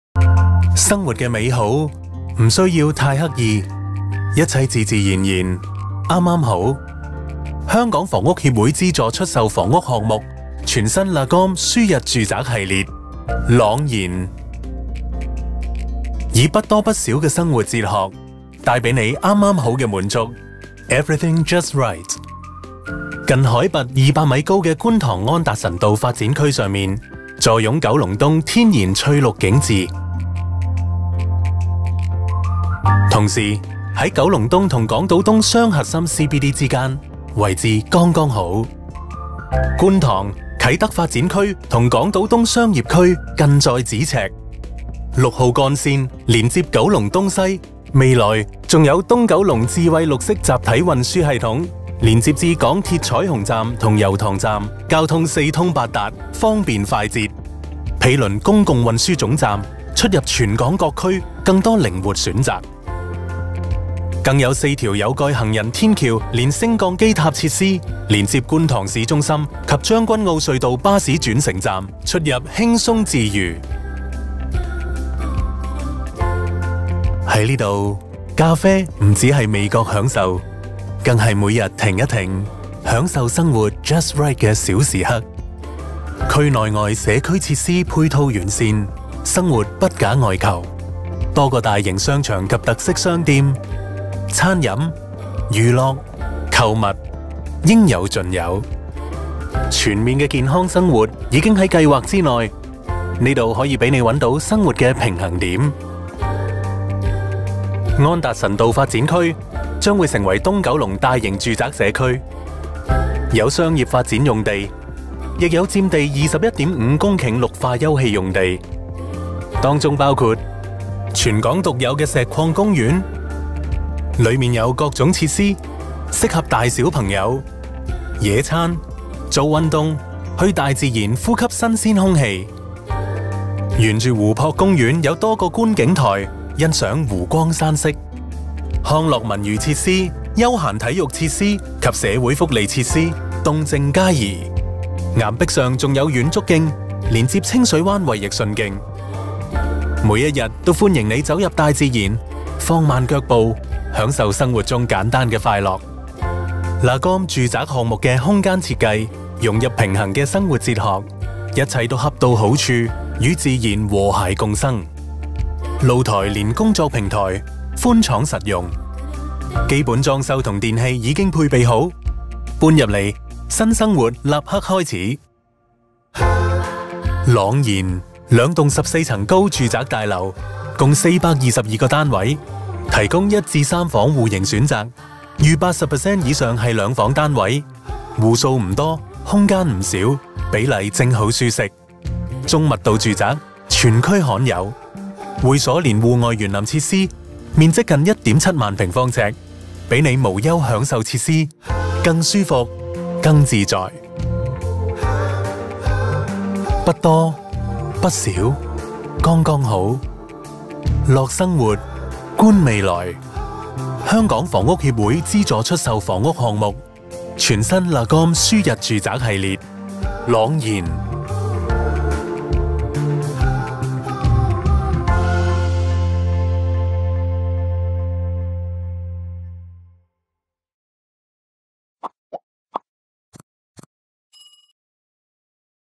房協資助房屋觀塘朗然 中低密度住宅 62折出售
香港房屋協會推出第5個資助房屋項目
位於觀塘安禧街18號
有2座，每座樓高14層，提供422個單位
實用面積介乎303至658平方呎
間隔由1房至3房
預計於2025年年底落成
朗然以市價62折出售
售價介乎255.5萬至645.6萬